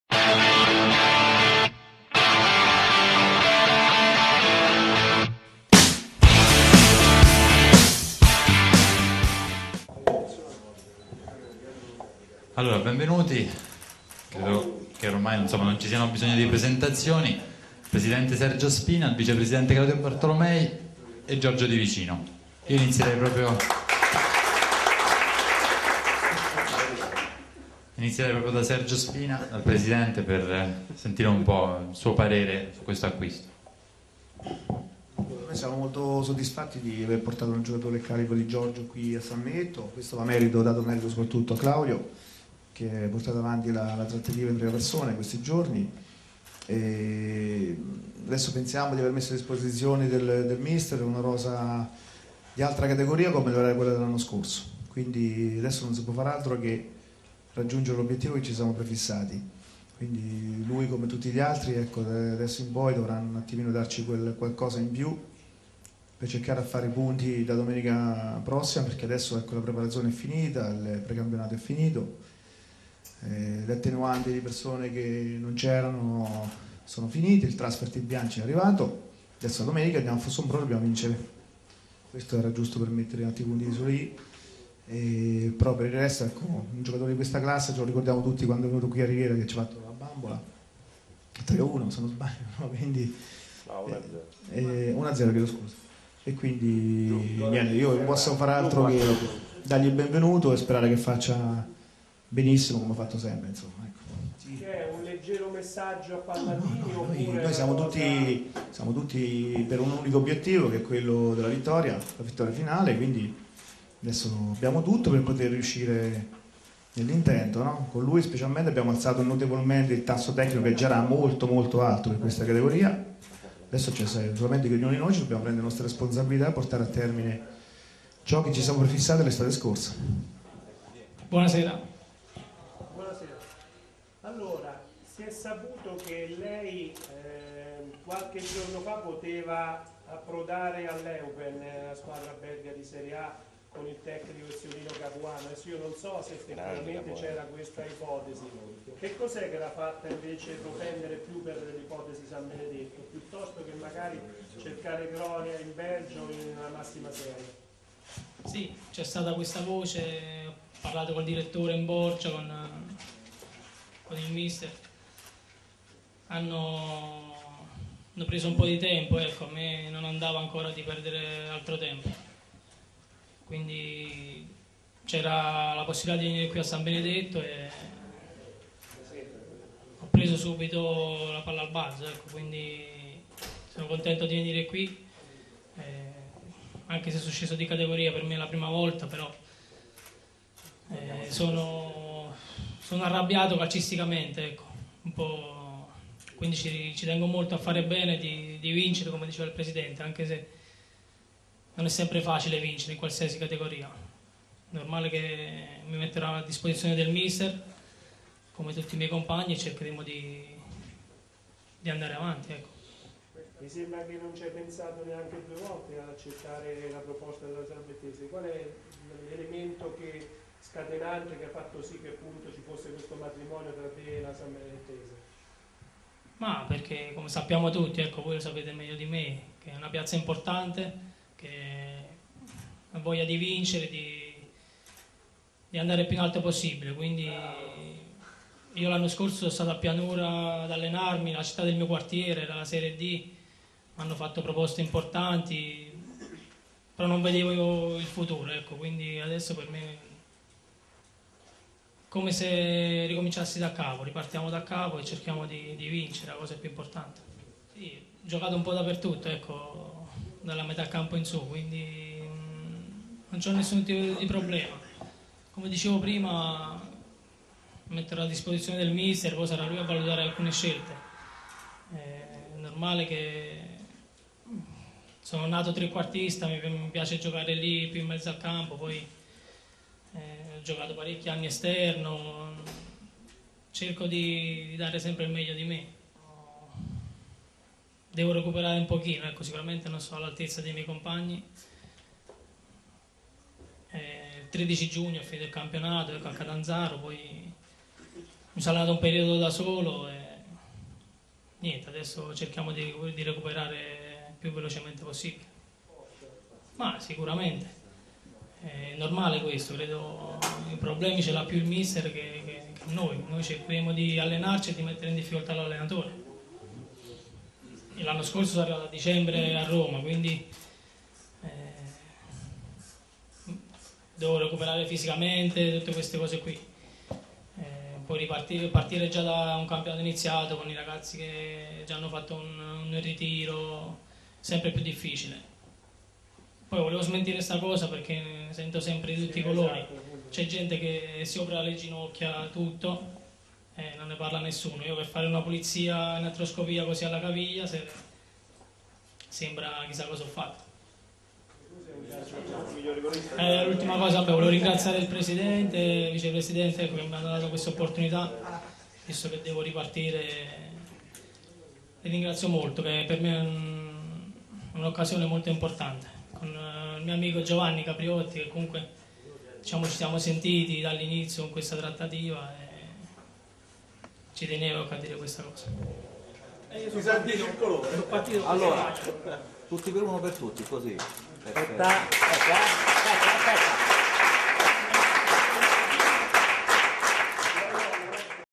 Giorgio Di Vicino
14.09.10 Presentazione del neoacquisto rossoblu', Giorgio Di Vicino, sala stampa Sabatino D'Angelo San Benedetto del Tronto